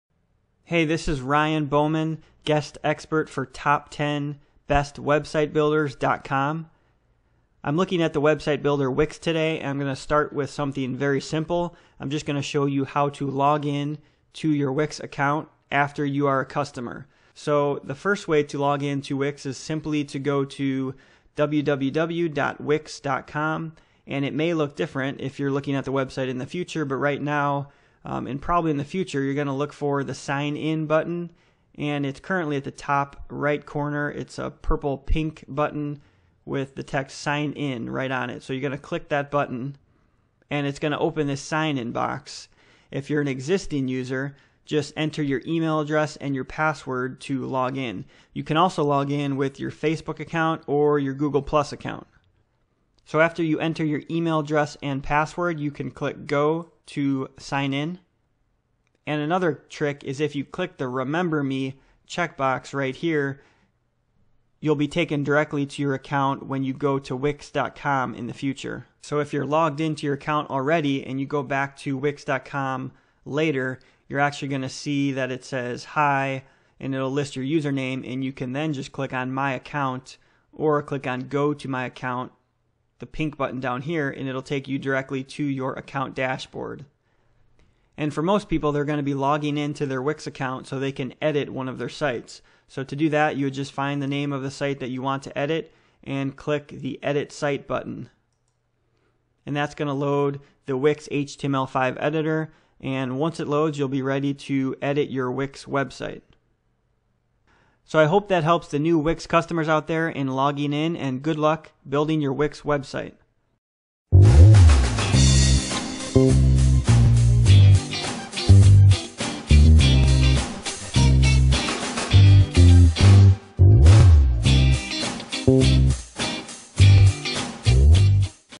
Wix login: a step-by-step guide to logging in to your Wix website
Find out how to login to Wix and edit your website with this video guide.

Looking for a website builder? Check out the Top 10 Best Website Builders